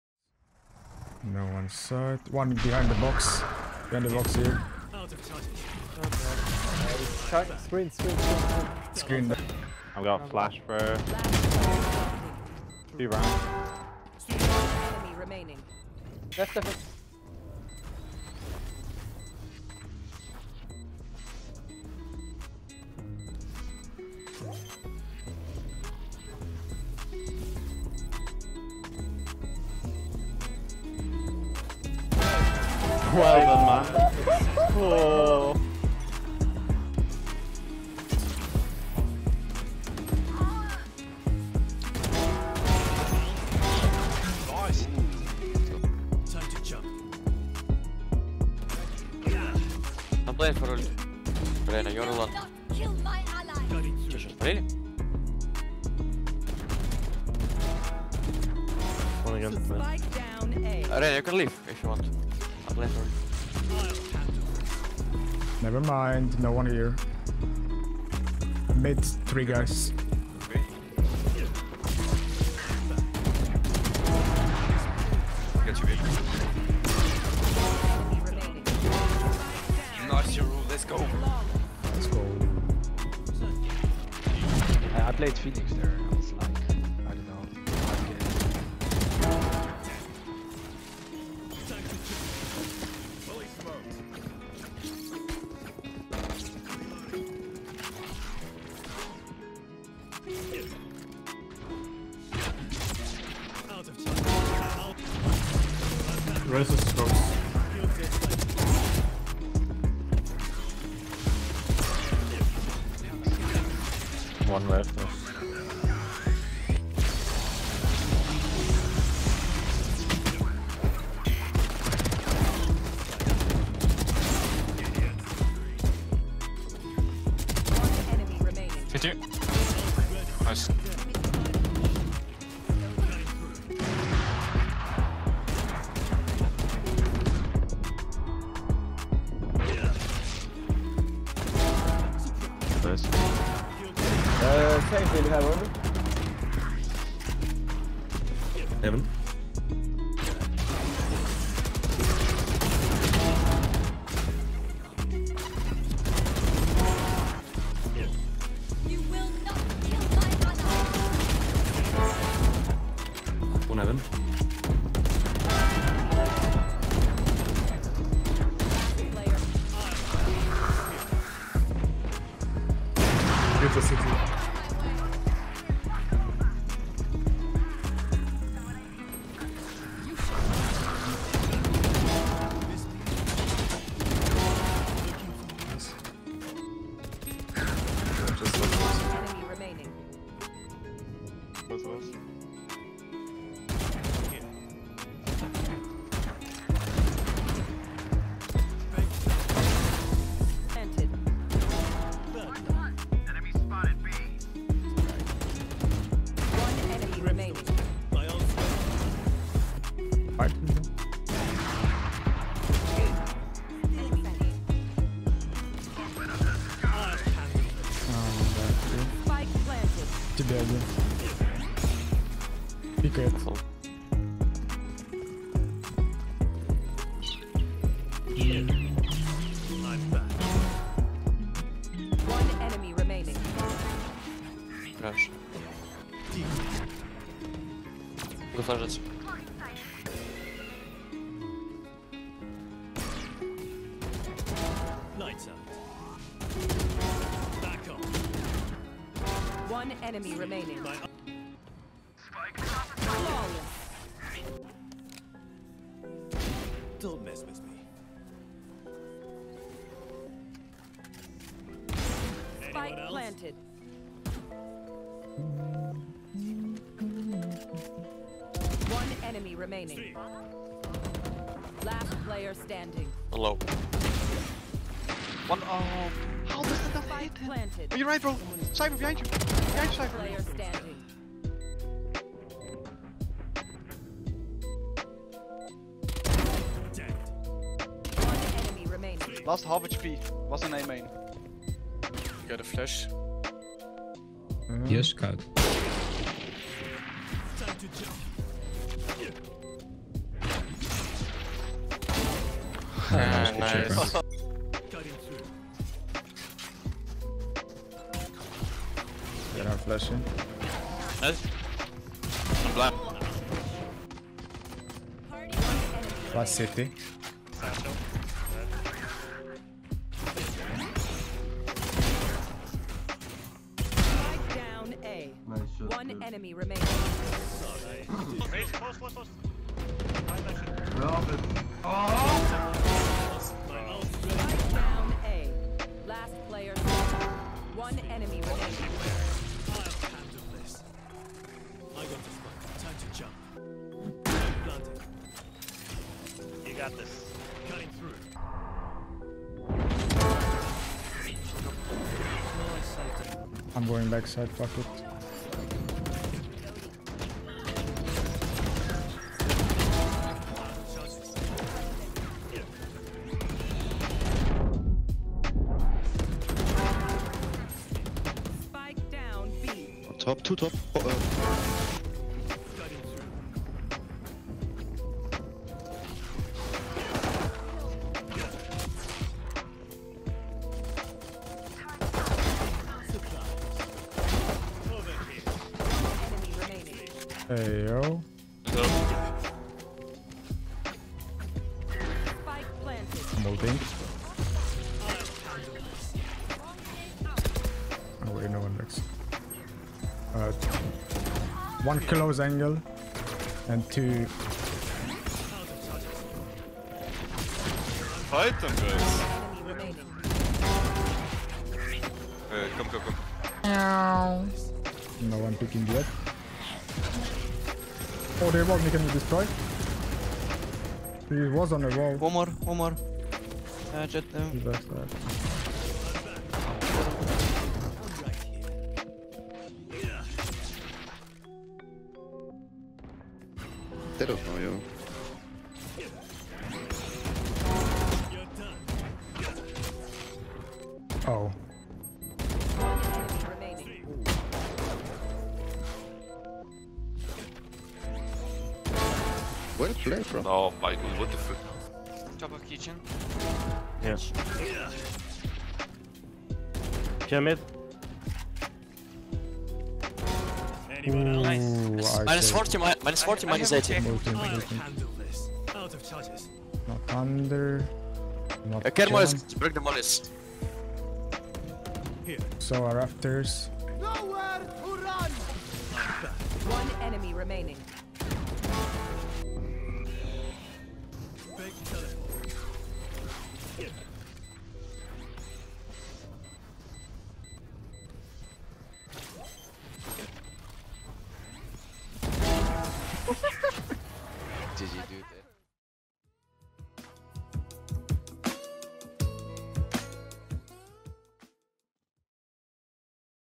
400 dpi 0.625 sens is a Cheat Code..
💜 Ty for all the likes, comments and subscribes, you really help me keep going and hella appreciated!
😈 Demon Yoru main gottcha with insane 200iq Yoru outplays, smart risk-taking, aggressive pushes, flash tp lineups, Oneway Minus Tempo Content Creator type shi plays.
400 dpi 0.625 sens is a Cheat Code..